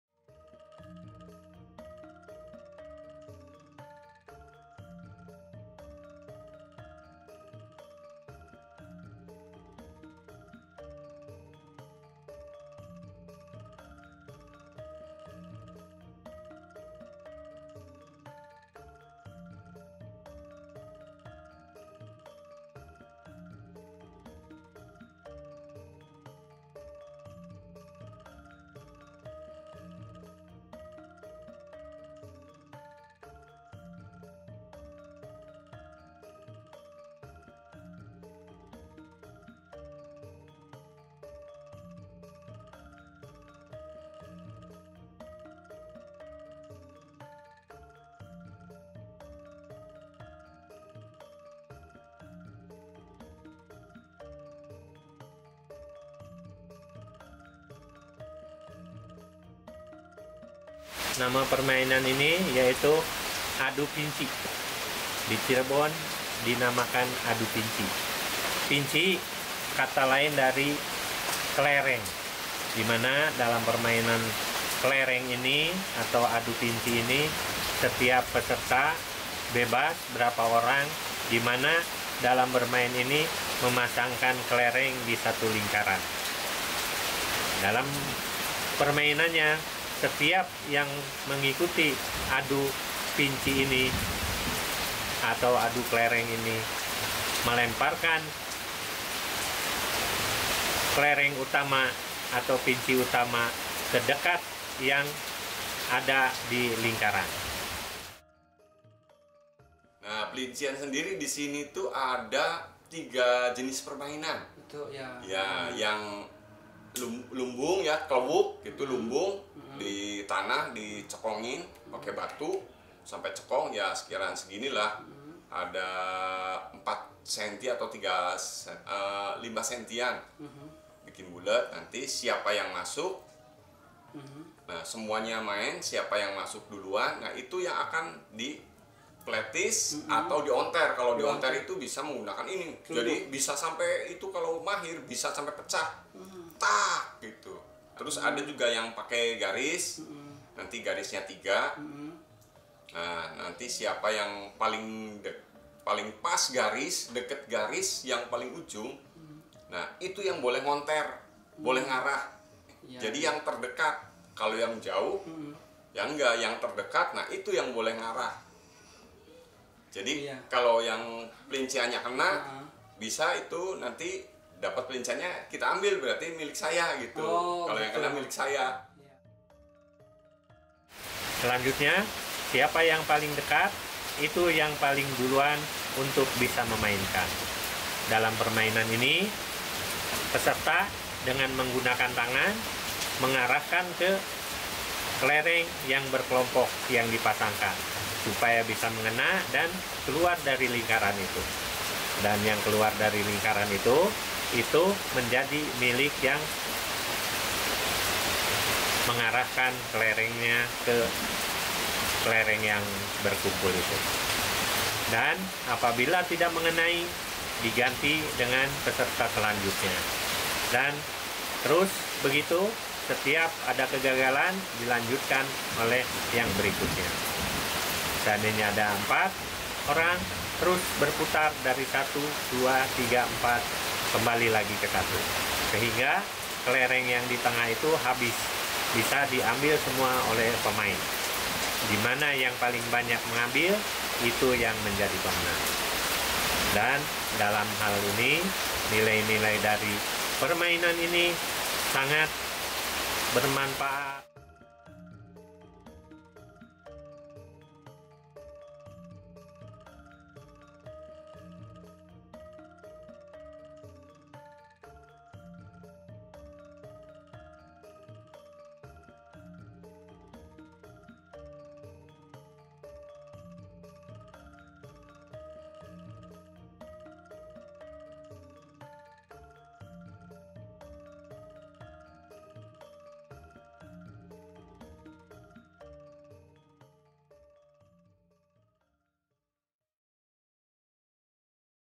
Permainan Tradisional Jawa Barat - Adu Plician
Adu Plician merupakan salah satu permainan tradisional yang ada di Jawa Barat.